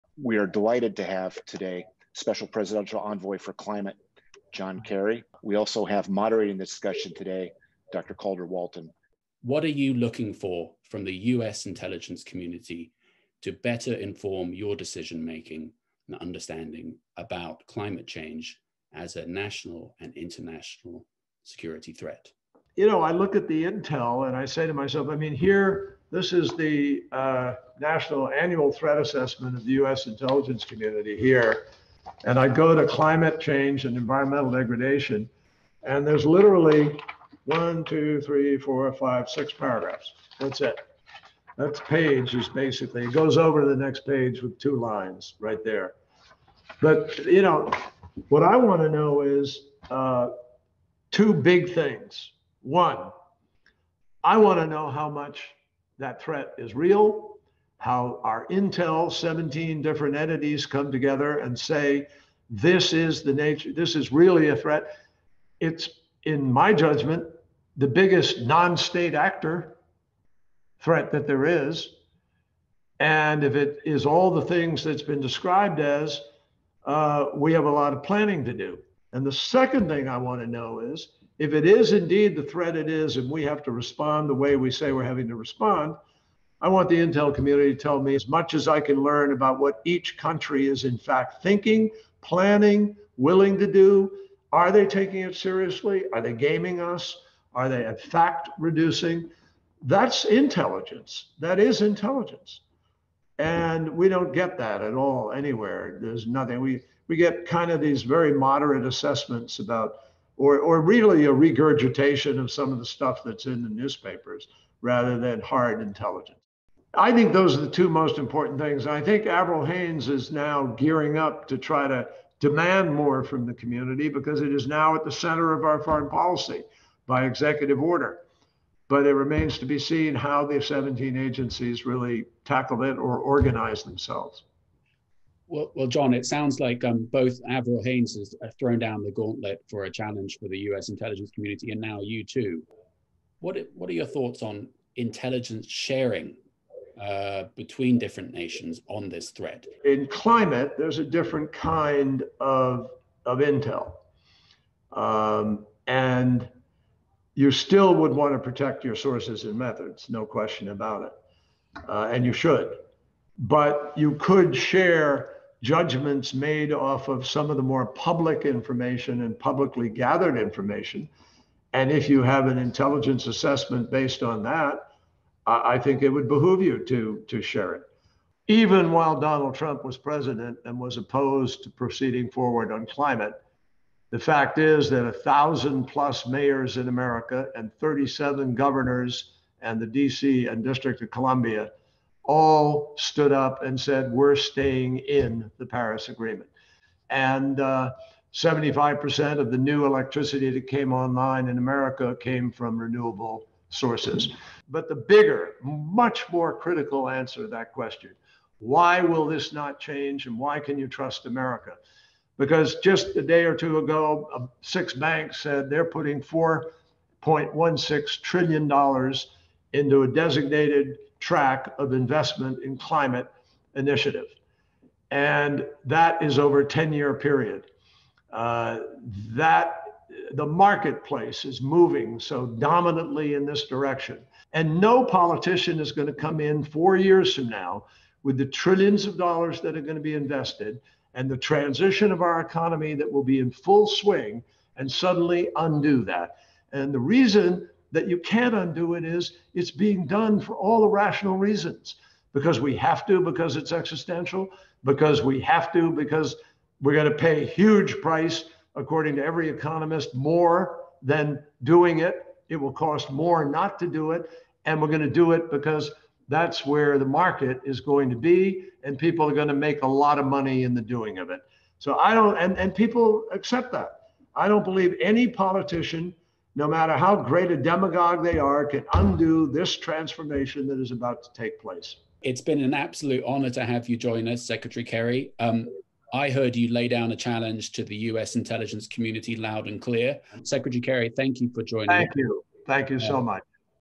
Secretary John Kerry Highlights – Climate Change, Intelligence, and Global Security Conference
Highlights from the keynote by Secretary John Kerry, Special Presidential Envoy for Climate.

Moderated by Dr. Calder Walton

Overview: Addressing climate change is now at the forefront of US national security and foreign policy strategies. How are the leaders of these communities thinking about and adapting to the challenges posed by climate change? What do the Biden administration and its international partners need from intelligence communities to meet these unprecedented challenges and how can this collaboration best be achieved?

Climate Change, Intelligence, and Global Security is a half-day conference co-sponsored by the Intelligence Project and the Environment and Natural Resources Program at Harvard Kennedy School's Belfer Center for Science and International Affairs along with the Center for Climate and Security.